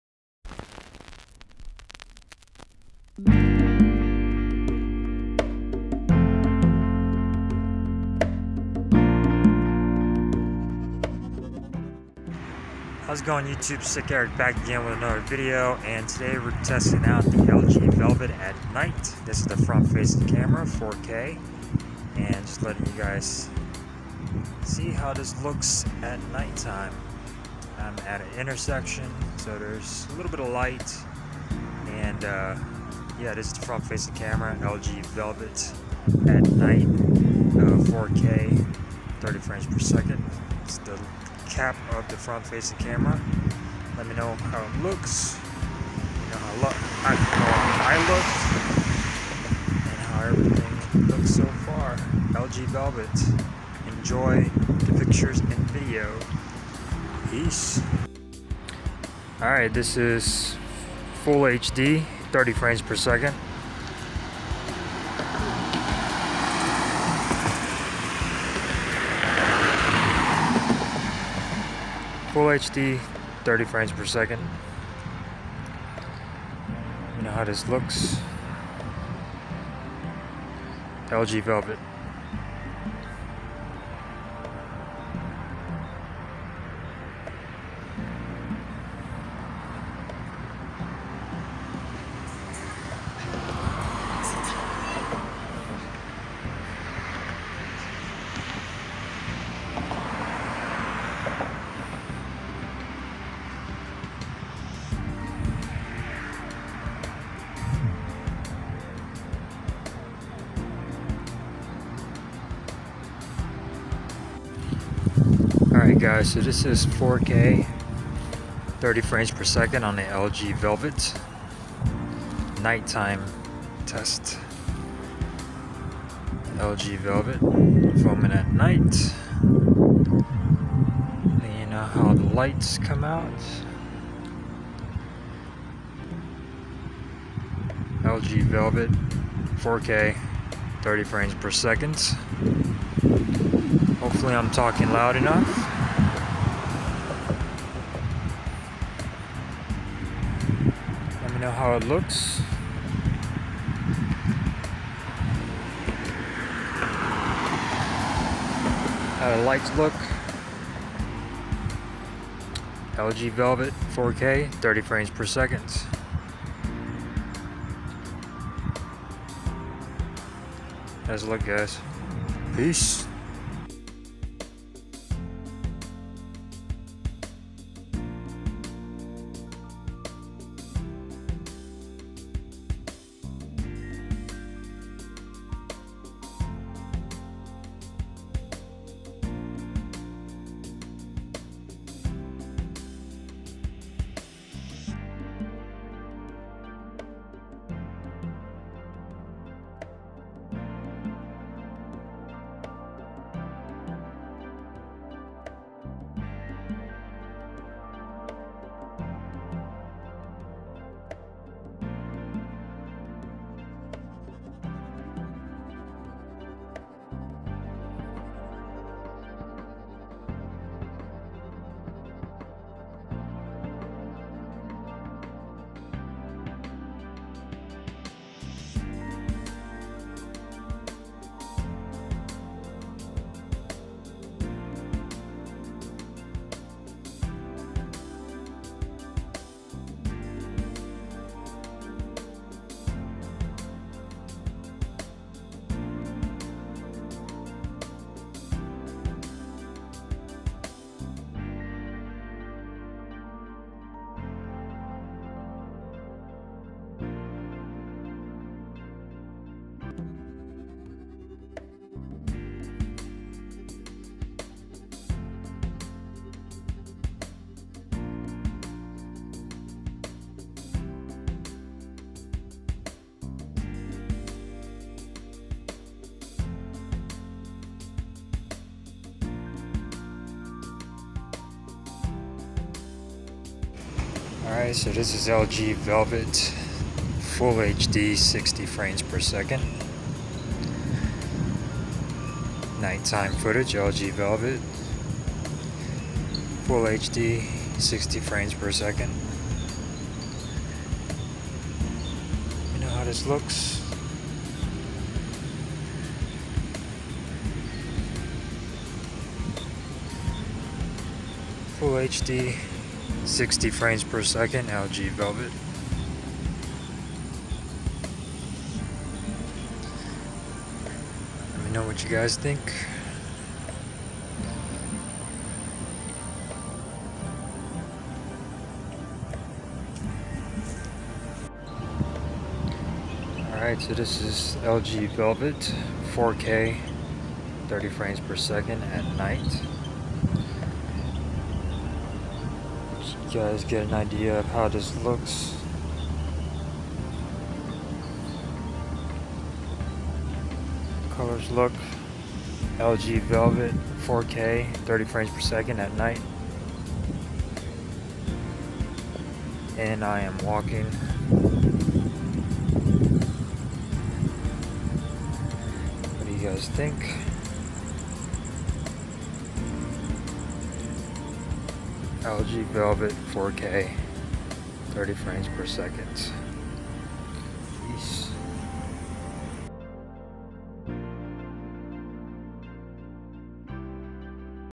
LG Velvet 5G (T-Mobile) | Night/Low Light Camera and Video Samples
These are some Photos and Video Samples taken at night with the LG Velvet 5G T-Mobile Variant.